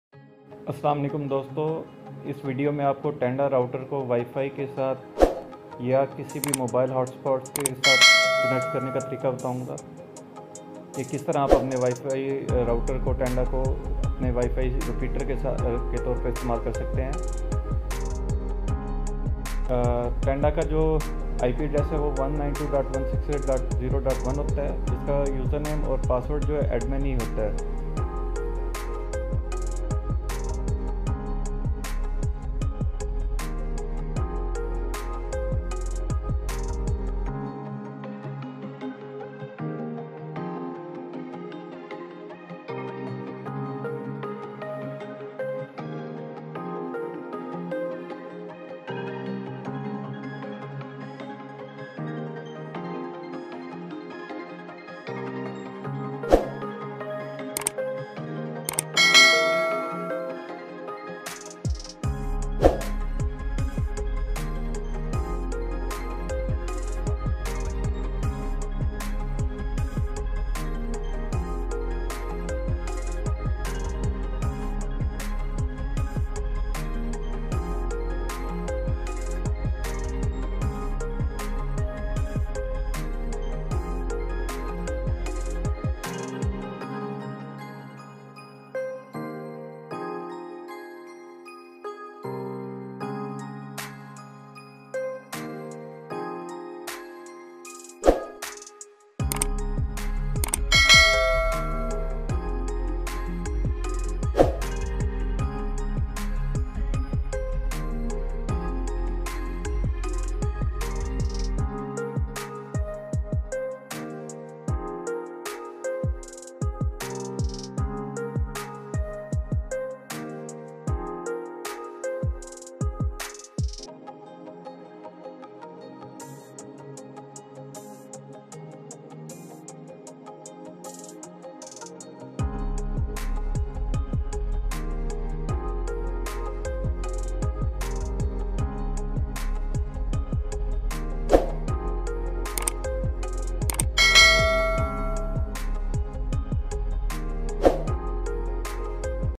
how to use tanda wifi router as repeater| tanda bridge setting
You can also use Tenda WiFi router in repeater mode to boost the WiFi signal without buying a WiFi repeater device. If your Main WiFi router does not give signal coverage to the whole home than using range extender mode in Tenda best option to increase signal IP to 100%.Tenda Wireless Universal Range Extender can be connected with any brand WiFi Access point which does not belong to Tenda brands.

Repeater mode also a smart way to connect your NVR, DVR, Network printer with a wired connection without laying physical cable. While using the Repeater mode device turn into a range extender to get internet wirelessly from Main internet router to repeat signal and internet speed.